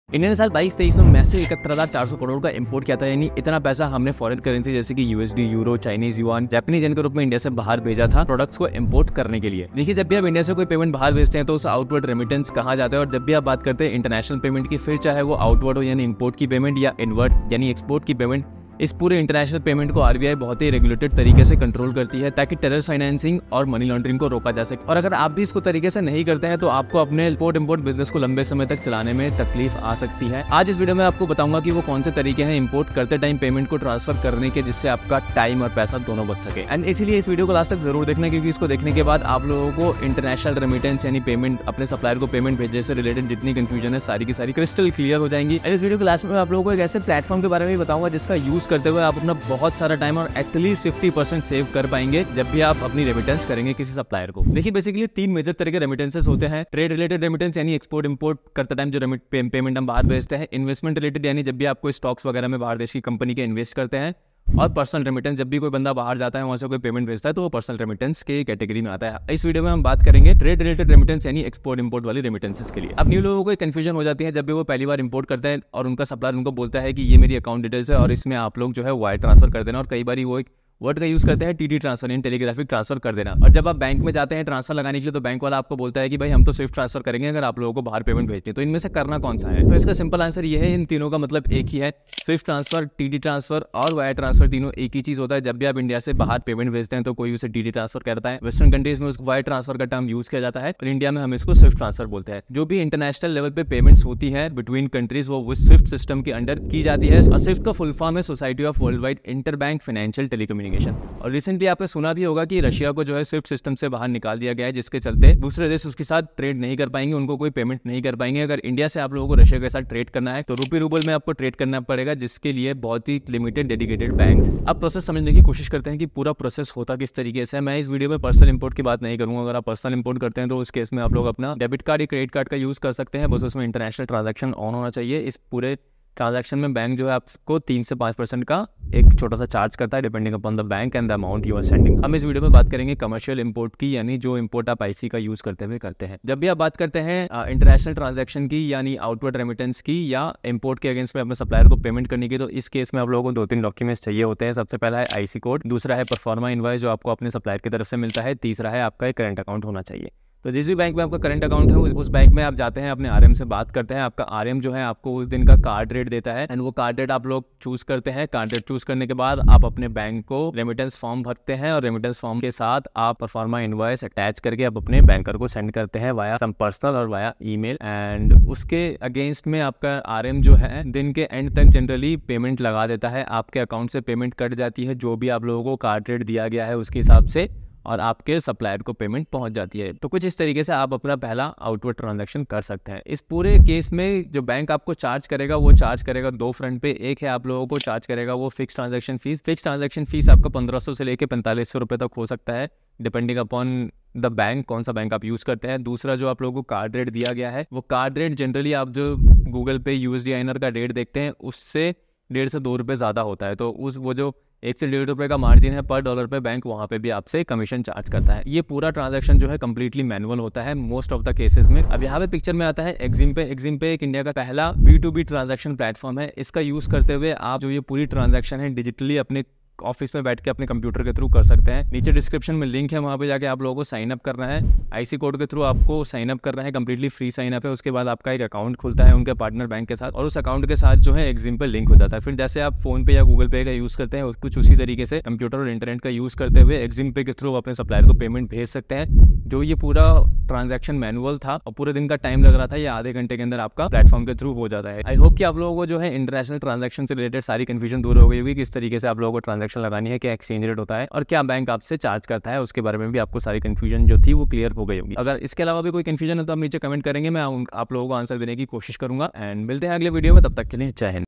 IB #6 | How To Send Payment To Supplier For Import | Alibaba | Wire | TT Transfer | Swift Transfer
Use the below link to get started with EximPe trade account for all your Export-Import Payments.

Every year, India imports goods worth billions of dollars, which leads to an outflow of foreign currency such as USD, EUR, Japanese Yen, and Chinese Yuan. However, with the increase in imports, the risk of fraud also rises. Many importers have faced situations where they made payments to new suppliers but either received no products or poor-quality items.

Every payment you make to your supplier is like a donation until you receive the goods. To regulate these international transactions and prevent money laundering and terrorism funding, India has strict regulations in place. Following these regulations is essential for the long-term operation of your import-export business.

In this video, we will discuss the correct ways to transfer payments while importing, saving you time and money. By the end of the video, you will have a clear understanding of international payment processes, eliminating any confusion or uncertainty.

We will also introduce you to a platform that can streamline your payment process, allowing you to complete it within 30 minutes while saving at least 50% of your time and effort.

Let's begin by focusing on trade-related remittances, specifically payments related to imports and exports. To make import remittances, you will need a current account and the bank account details of the recipient.

There is often confusion regarding wire transfers, TT (Telegraphic Transfer), or Swift transfers. In reality, they all refer to the same thing. Your supplier may use different terms, but in India, it is commonly referred to as Swift transfer. Swift stands for "Society for Worldwide Interbank Financial Telecommunications," and it is the system through which international funds are transferred.

For swift transfers, you will need a current account and the necessary export-import documents.

In this video, we will explain the process in detail, focusing on commercial imports that involve using your Importer Exporter Code (IEC). We won't cover personal imports, as they can be done using your debit or credit card with international transaction capabilities.

Join us on this journey to understand the proper payment methods for your commercial imports.

Contact Mr Sujeet for Customs Clearance